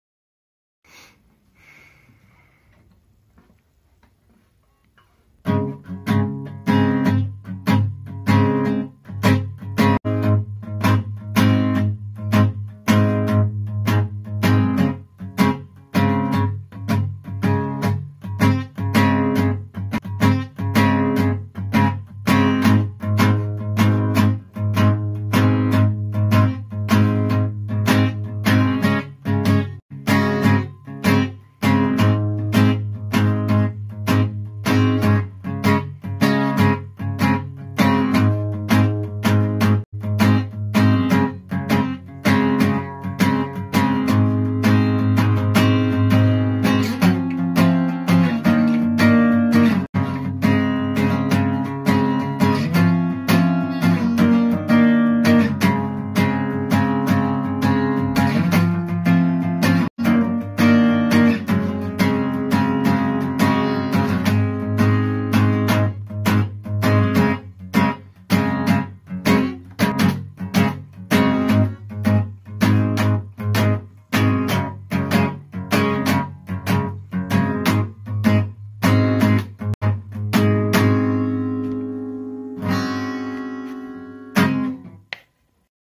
My first song (Demo)
This is an unfinished bit of something I wrote.